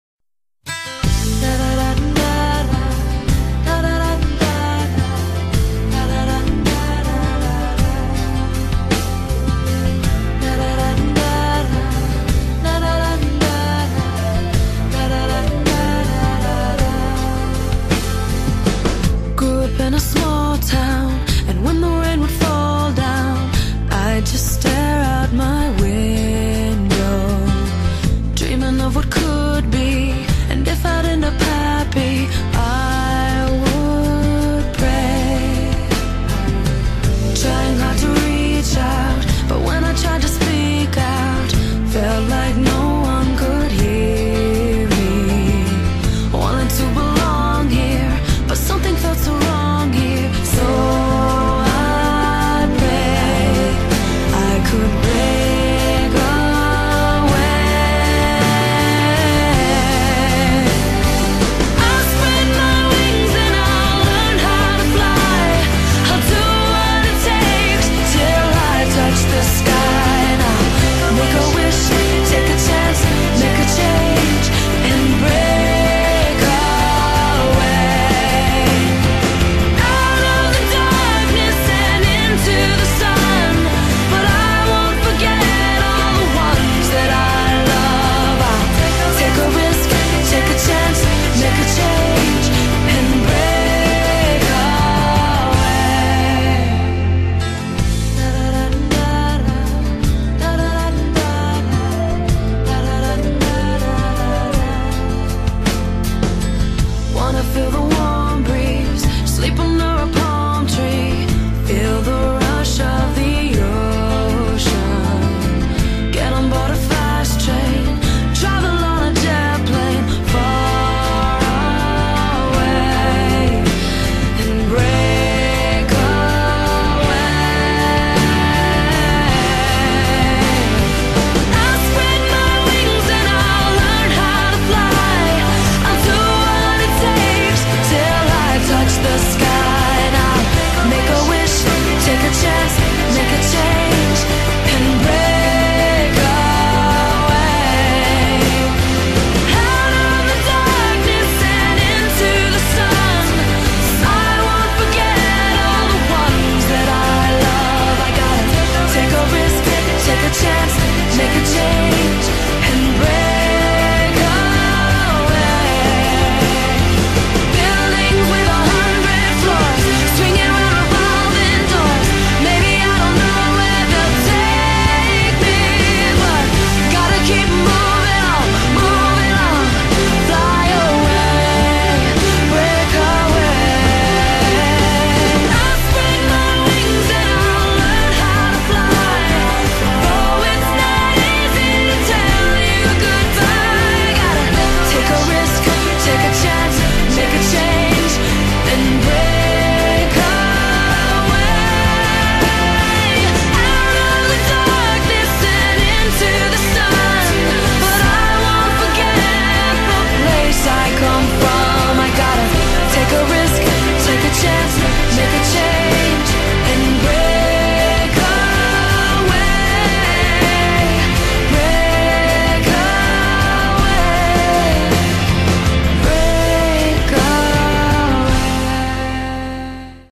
Teddy O'Connor Tribute
a tribute to the amazing pony who was euthanized on May 28, 2008 as a result of an injury sustained at Karen and David O'Connor's barn. i tried to make a nice video with the music that i had, so please, nice comments only. and i know its a little late but i still wanted to make it.